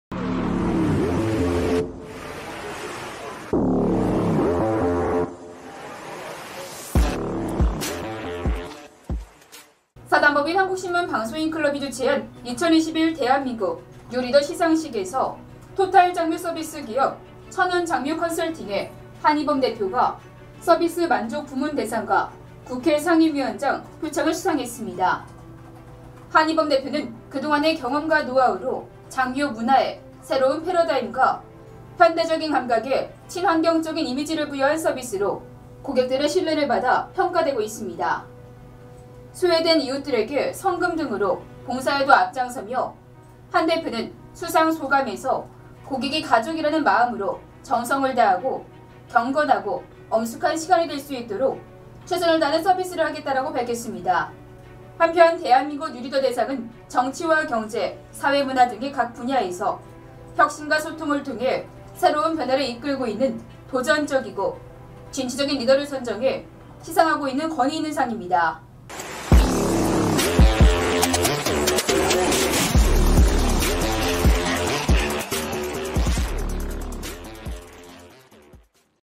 천운장묘컨설팅 한의범대표 '2021 대한민국 뉴리더대상' 서비스만족부문대상 국회 상임위원장 표창 수상
(사)한국신문방송인클럽이 주최한 2021 대한민국 뉴리더 시상식에서 토탈장묘 서비스 기업 천운장묘 컨설팅의 한의범대표가 서비스만족부문 대상과 국회 상임위원장 표창을 수상했습니다.

한대표는 그동안의 경험과 노하우로 장묘문화에 새로운 패러다임과 현대적인 감각의 친환경적인 이미지를 부여한 서비스로 고객들의 신뢰를 받아 왔다고 평가되고 있습니다.

소외된 이웃들에게 성금등으로 봉사에도 앞장서는 한대표는 수상소감에서 고객이 가족이라는 마음으로 정성을 다하고 경건하고 엄숙한 시간이 될수 있도록 최선을 다하는 서비스를 하겠다고 밝혔습니다.

한편 「대한민국 뉴리더 대상」은 정치·경제·사회·문화 등 각 분야에서 혁신과 소통을 통해 새로운 변화를 이끌고 있는 도전적이고 진취적인 리더를 선정해 시상하고 있는 권위있는 상이다.